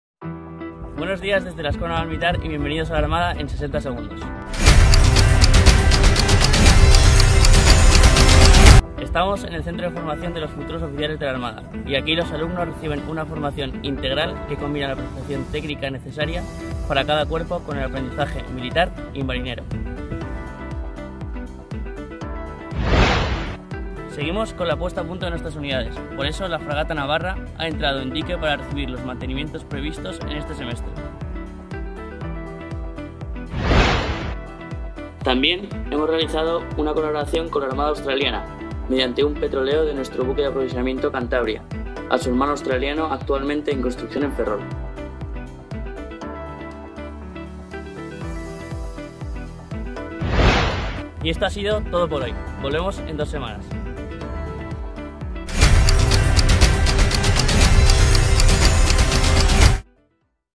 La Armada en 60 segundos Cap. 10
Bienvenidos una semana más a “La armada en 60 segundos”.

El buque de aprovisionamiento Cantabria, la fragata Navarra y la EscuelaNavalMilitar son nuestros protagonistas en esta edición.

No te quedes sin conocer los detalles de nuestra actualidad.